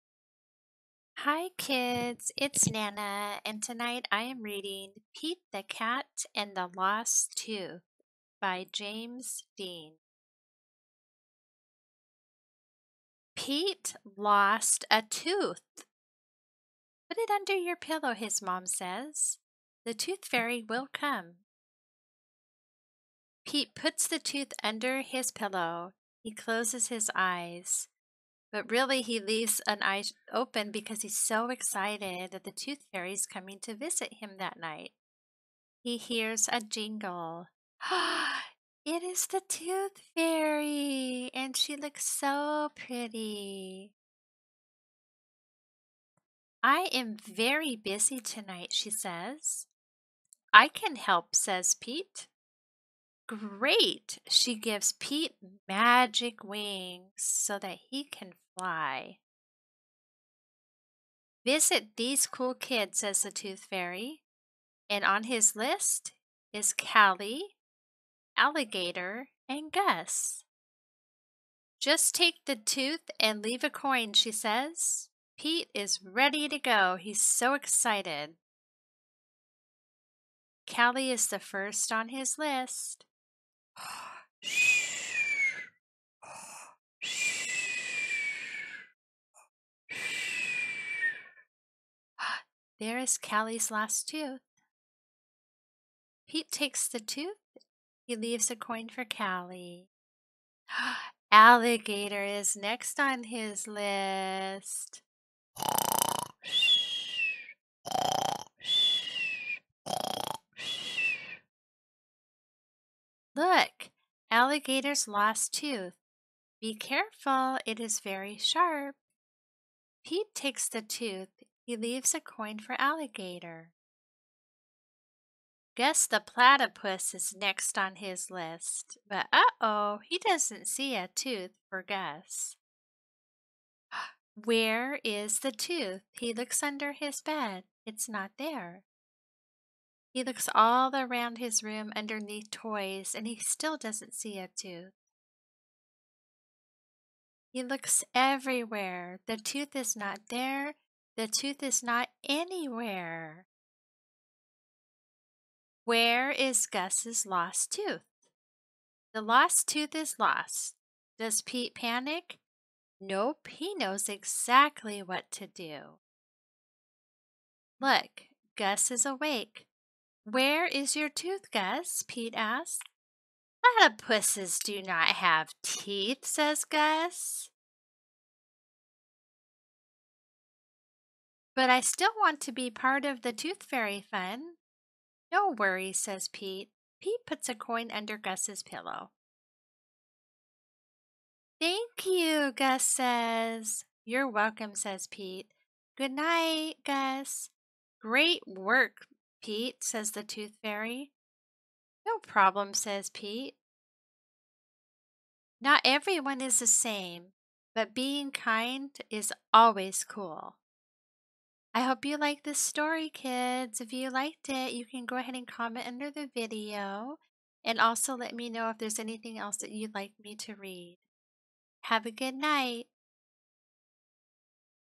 StoryTime with Nana
Pete the Cat and the lost tooth.